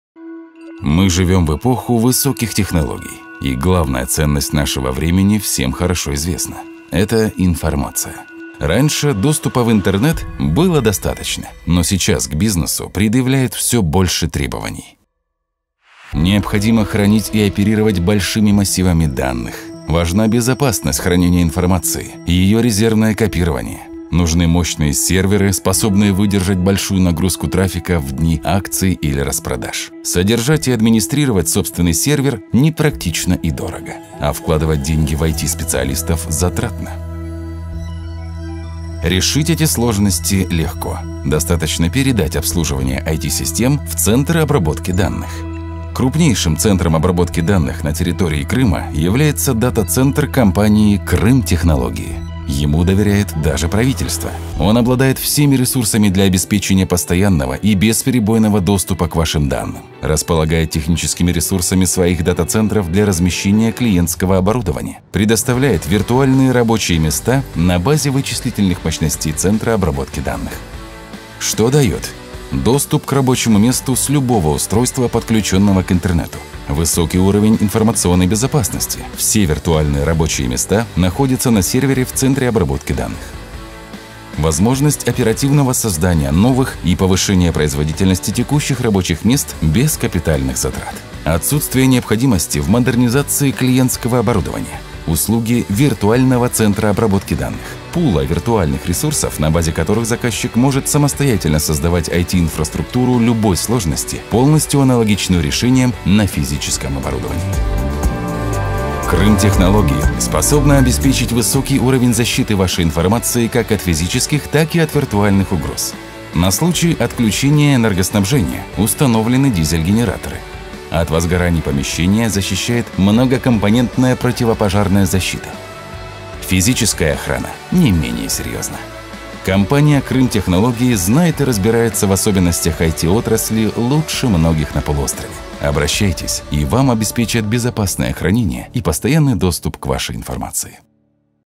Центр обработки данных АО "Крымтехнологии"
Крупнейшим центром обработки данных на территории Республики Крым, является дата-центр компании Крымтехнологии. Нам доверяет даже правительство.
Обращайтесь к нам, и мы обеспечим Вам безопасное хранение и постоянный доступ к вашей информации.